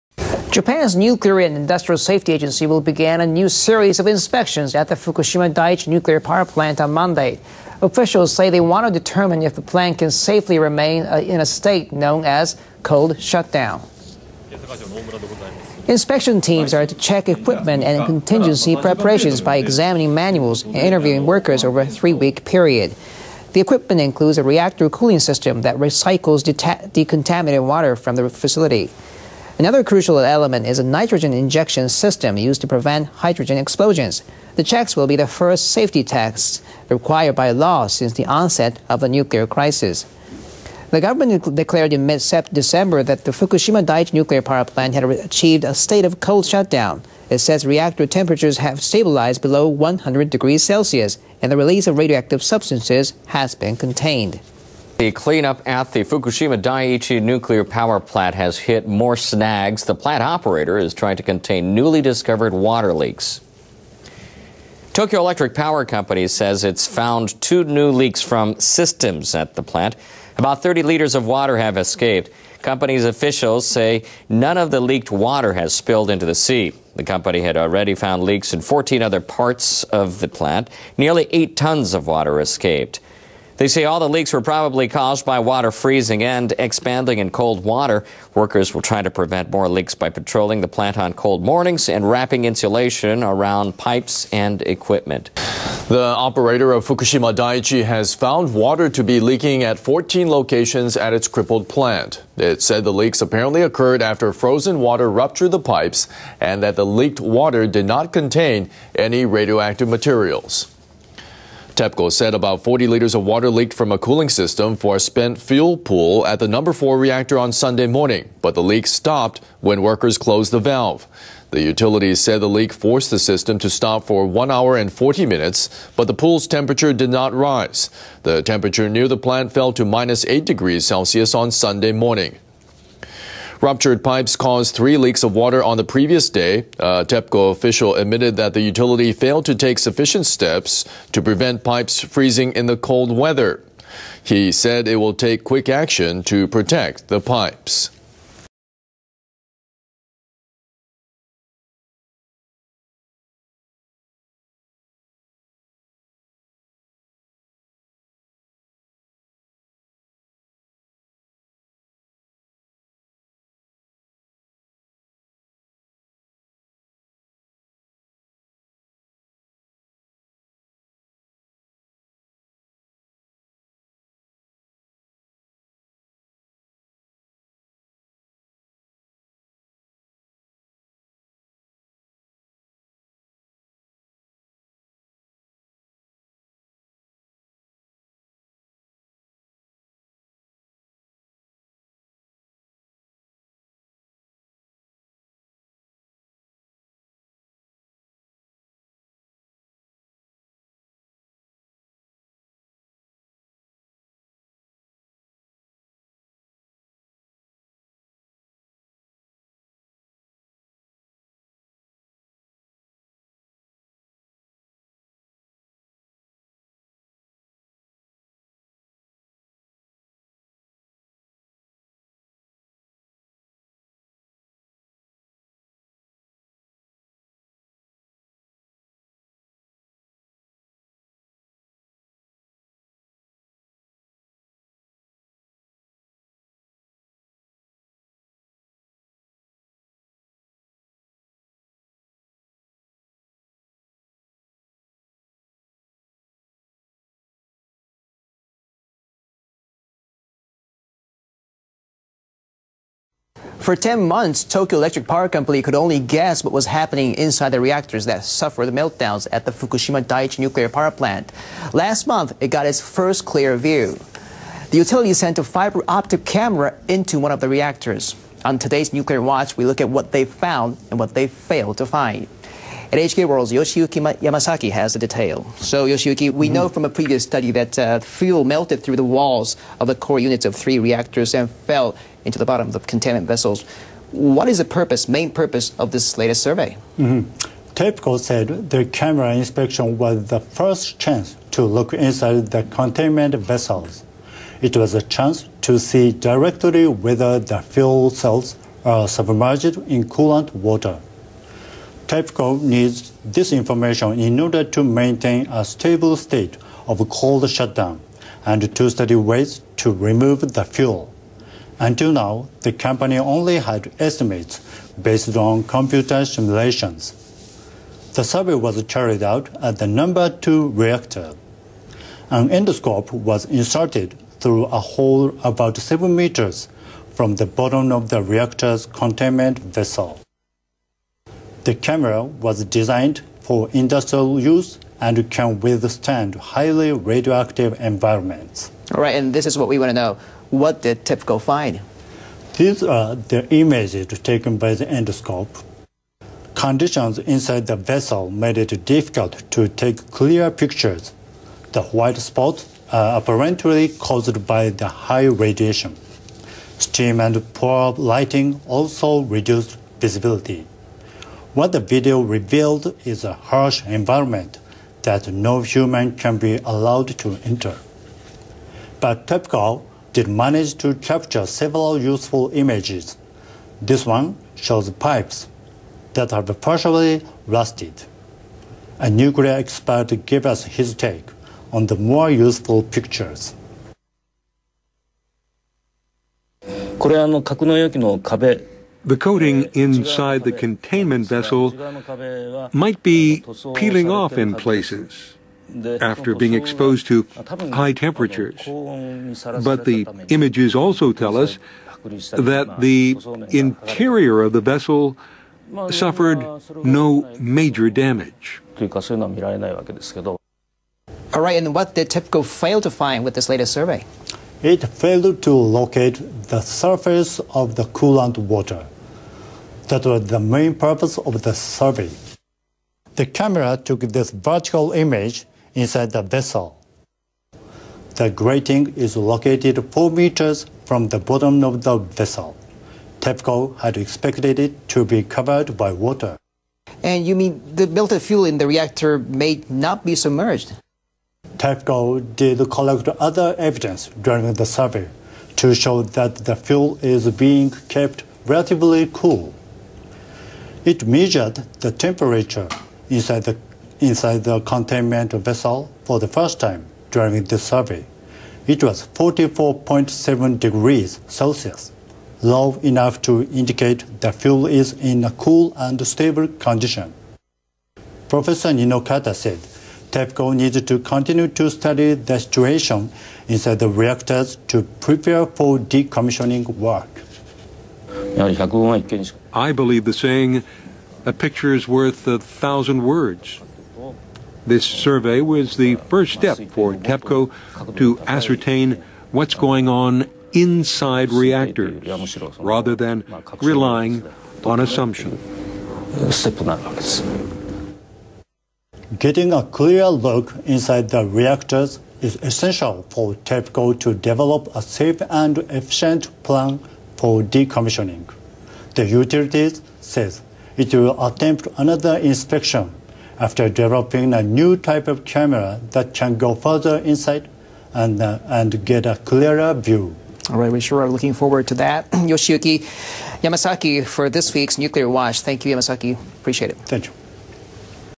Fukushima Nuclear Plant Failures/Updates, Jan 30 - Feb 3 2012
NHK is Japanese mainstream media -- critical thinking is strongly advised!

See also:
Must Read ARTICLES-
State Health Dept. Monitoring Shoreline For Radioactive Waste